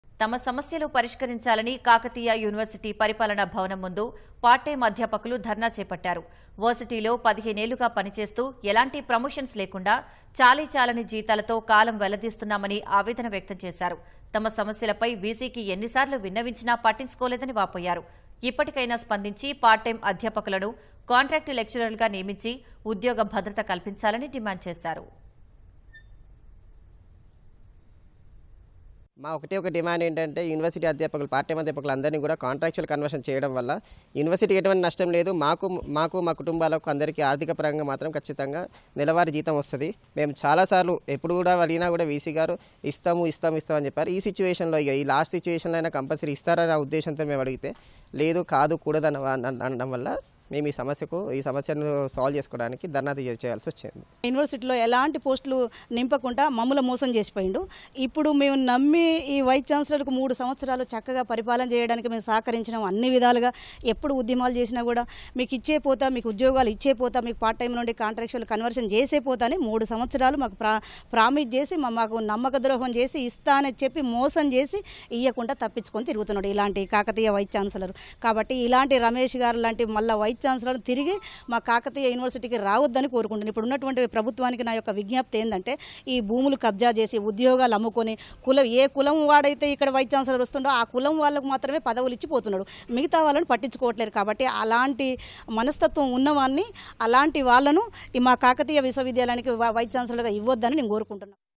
Part Time Teachers Dharna At Kakatiya University | కాకతీయ యూనివర్సిటీలో పార్ట్ టైం అధ్యాపకులు ధర్నా
తమ సమస్యలు పరిష్కరించాలని కాకతీయ యూనివర్సిటీ పరిపాలన భవనం ముందు. పార్ట్ టైం అధ్యాపకులు ధర్నా చేపట్టారు . వర్సిటీలో 15 ఏళ్లుగా పనిచేస్తూ ఎలాంటి ప్రమోషన్స్ లేకుండా... చాలీచాలని జీతాలతో కాలం వెళ్లదీస్తున్నామని ఆవేదన వ్యక్తం చేశారు . తమ సమస్యలపై VCకి ఎన్నిసార్లు విన్నవించినా .. పట్టించుకోలేదని వాపోయారు. ఇప్పటికైనా స్పందించి పార్ట్ టైం అధ్యాపకులను కాంట్రాక్ట్ లెక్చరర్లుగా నియమించి ఉద్యోగ భద్రత కల్పించాలని డిమాండ్ చేశారు.